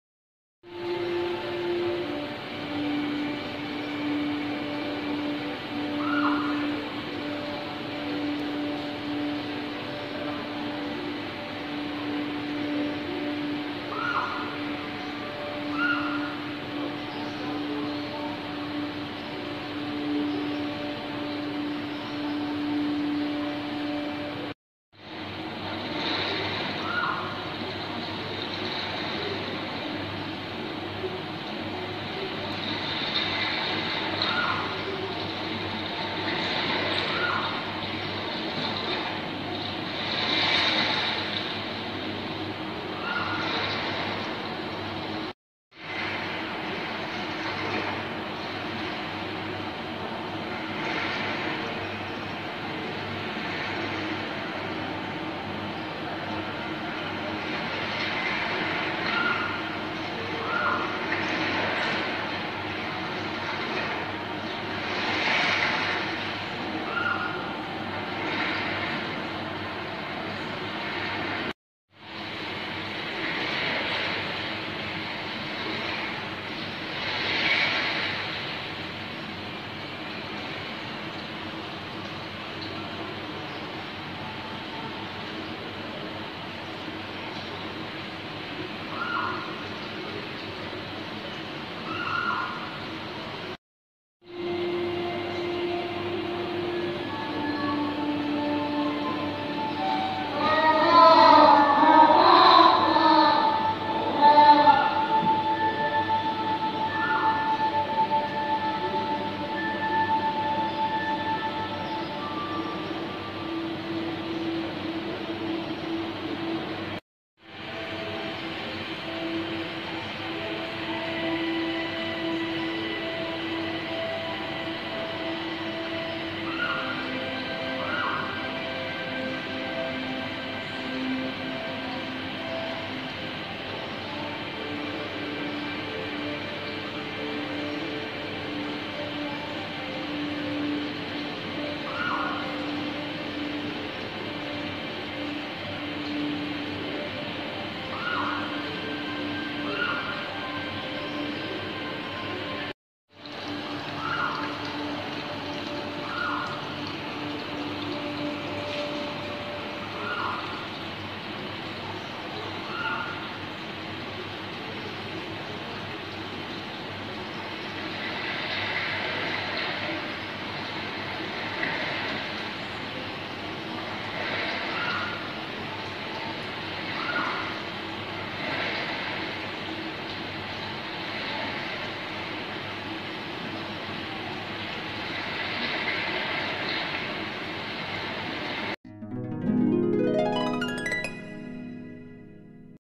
Потрясные пингвины 😍🐧🐧
Копошатся, ворчат, деловито прохаживаются - я долго не могла оторваться от пингвинов в океанариуме. К чему я это? Ах да, мне они точно нужны! 😍🐧🐧

🌠 Мой сайт, где будет много интересного, в том числе мой Дневник похудения